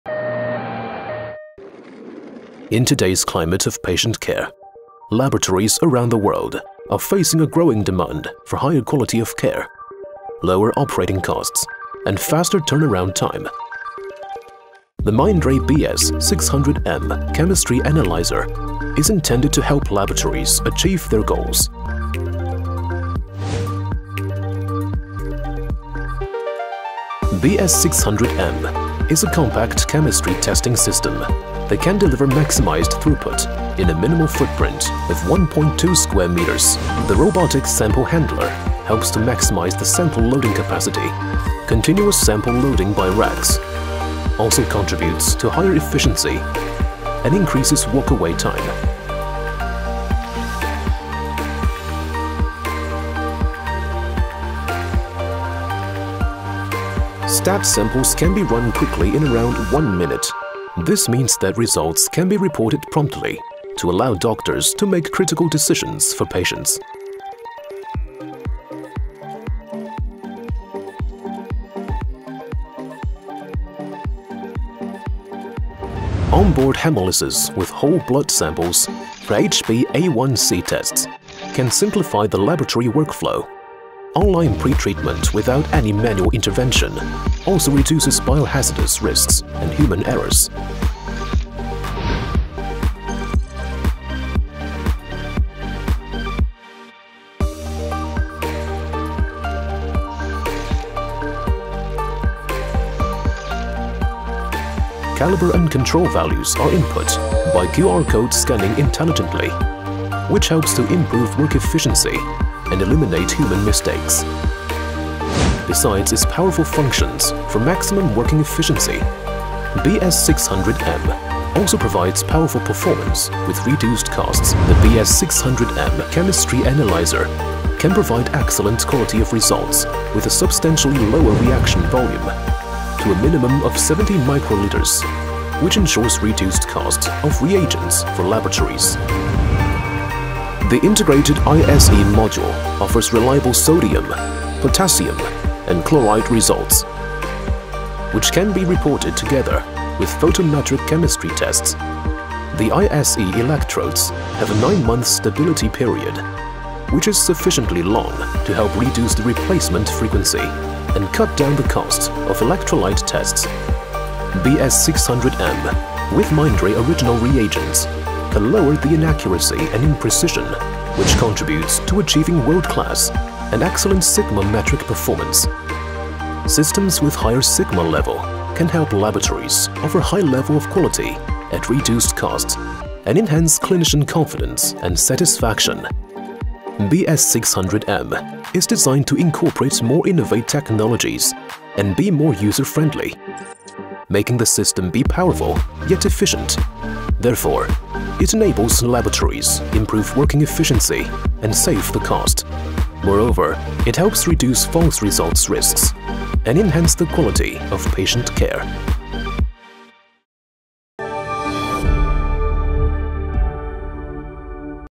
BS-600M Product Video | Mindray Biochemistry Analyzer | New Launch | Excellent Accuracy & Precision
Mindray has launched the new BS-600M, a powerful yet efficient chemistry analyzer designed to empower medium-volume laboratories with increased productivity, efficiency and reliability.

With a compact footprint of only 1.2 square meters and a throughput of 600 T/H (up to 800 T/H with ISE), the BS-600M chemistry analyzer has among the highest levels of throughput per square meter. It maximizes productivity for laboratories in a minimum amount of space. BS-600M allows continuous sample loading by racks and flexible STAT priority, which enables higher efficiency and prolonged walk-away time. Having high Sigma metric performance, it can help laboratories deliver high levels of testing quality, reduce QC measurements, simplify QC rules, and shorten turn-around time. These advantages enable significant reductions in troubleshooting work and consumption of QCs and reagents.

BS-600M features on-board hemolysis with whole blood for HbA1c tests. This eliminates the need for manual pretreatment, improving testing efficiency and reducing biohazardous risks and human errors. Mindray’s HbA1c assay on BS-600M can achieve excellent accuracy and precision. The enzymatic HbA1c method can also effectively prevent interferences from Hb variants and ensure no interference from other Hb derivatives. This means that reliable results can be obtained at the first measurement, without requiring further confirmations. Additionally, BS-600M requires much reduced reaction volumes for HbA1c tests, which contributes to around 30% lower cost per test. BS-600M also integrates a brand-new ISE solution, which reduces both the frequency of electrode replacement and the consumption of ISE reagents. As the result, the cost per test for ISE is reduced by around 35%. Moreover, ISE tests on BS-600M exhibit top levels of accuracy and precision in the field of ISE detection.

BS-600M also incorporates several other powerful functions, including intelligent detection, alarming and auto-rerun of abnormal reactions, fast input of QC & CAL target values via barcode scanning, a user-friendly operation interface, and breathing LEDs that indicate the status of the analyzer. All these powerful functions allow BS-600M to help medium-sized laboratories maximize their working efficiency and cost efficiency, and provide an amazing user experience for laboratory technicians. The performance advantages of BS-600M allow it to offer highly reliable results at reduced costs and to enhance clinician confidence and satisfaction.

For any support, please contact Mindray India using the below information:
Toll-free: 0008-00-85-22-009